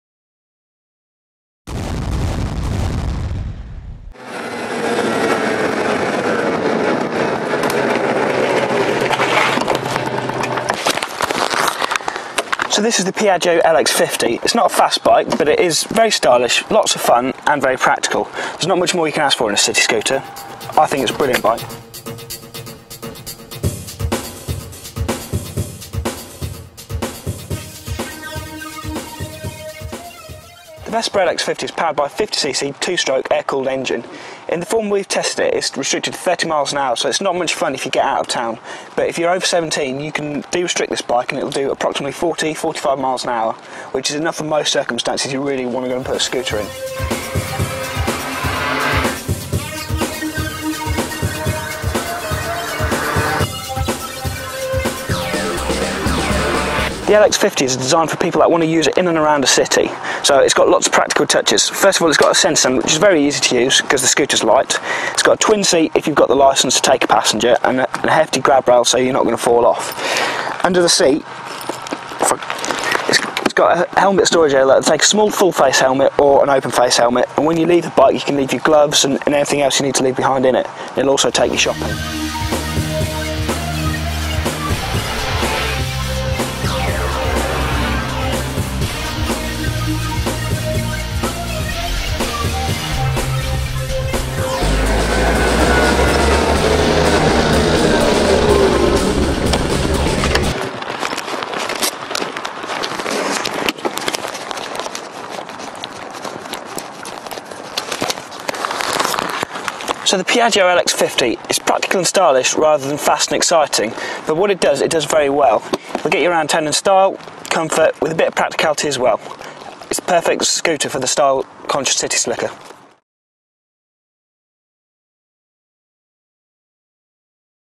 Piaggio Vespa LX50 review
It may not be fast, but the Piaggio Vespa LX50 is very stylish, lots of fun and very practical. Watch the video to see the Vespa in action and hear about some of the features that make it the perfect scooter for image conscious city types.